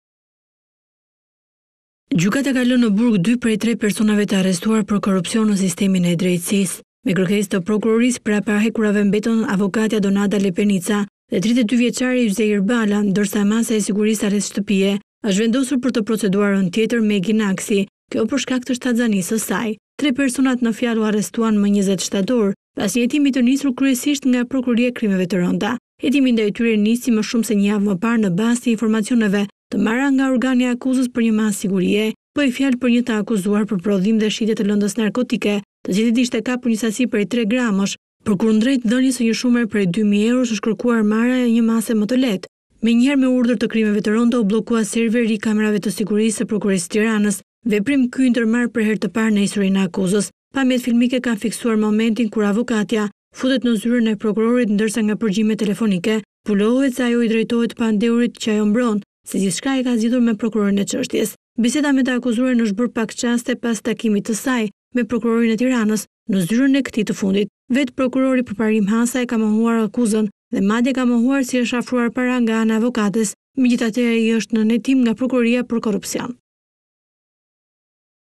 Ora News - 2 mijë euro për masën e sigurisë, avokatja dhe një i arrestuar mbeten në burg
Gjykata ka lënë në dy burg prej tre personave të arrestuar për korrupsion. Prapa hekurave mbeten avokatja Donata Lepenica dhe 32 vjeçari Yzeir Bala, ndërsa masa e sigurisë "arrest shtëpie" është vendosur për Megi Naksi, për shkak të shtatzënisë së saj.

Tre personat në fjalë u arrestuan më 20 shtator.

Hetimi ndaj tyre nisi më shumë një javë me parë në bazë të informacioneve të marra nga prokuroria për një masë sigurie.

Bëhej fjalë për një të akuzuar për prodhim dhe shitje të lëndës narkotike, të cilit i ishte kapur një sasi prej 3 gramësh.

Përkundrejt dhënies së një shume prej 2 mijë eurosh, është kërkuar marrja e një mase detyrim paraqitje.

Prej 4 ditësh, Krimet e Rënda kanë bllokuar serverin e kamerave të sigurisë së Prokurorisë së Tiranës, veprim ky i ndërmarrë për herë të parë në historinë e akuzës.

Pamjet filmike kanë fiksuar momentin kur avokatja futet në zyrën e Hasës.

Ndërsa nga përgjimet telefonike zbulohet se avokatja i drejtohet të pandehurit që ajo mbron se, "gjithçka e kam zgjidhur me prokurorin".

Biseda me të akuzuarin është bërë pak çaste pas takimit të saj me prokurorin e Tiranës, në zyrën e këtij të fundit.

Vetë Hasa e ka mohuar akuzën dhe madje ka mohuar se i është afruar para nga ana e avokates.